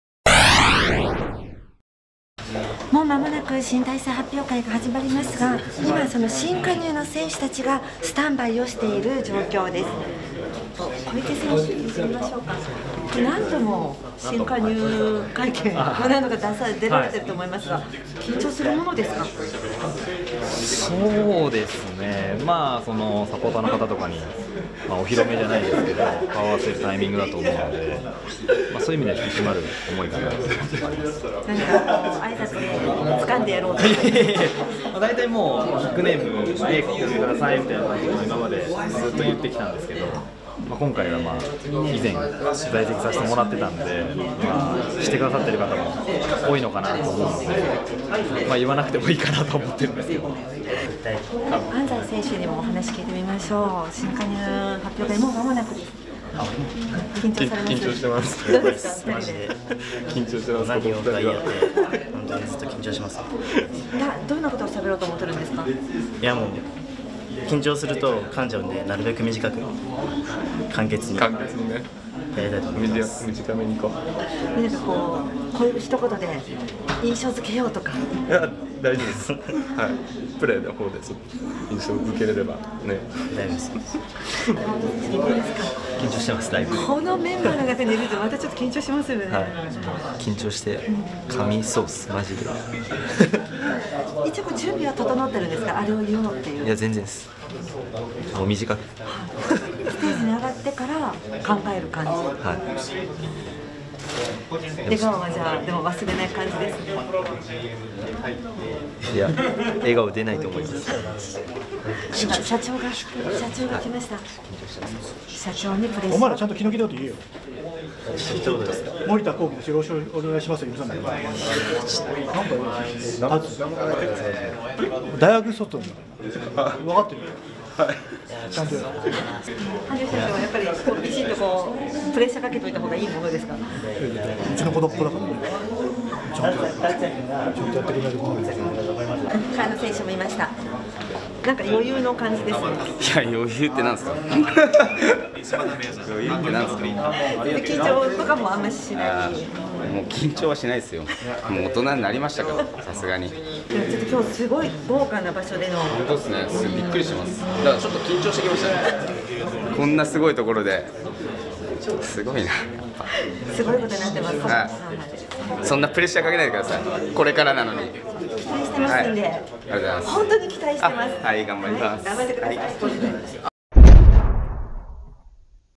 VERDY TV／INSIDE 新体制 part.1
新加入選手登壇直前の選手たちを直撃！緊張しまくりの昇格選手たちに羽生社長がプレッシャーをかける!?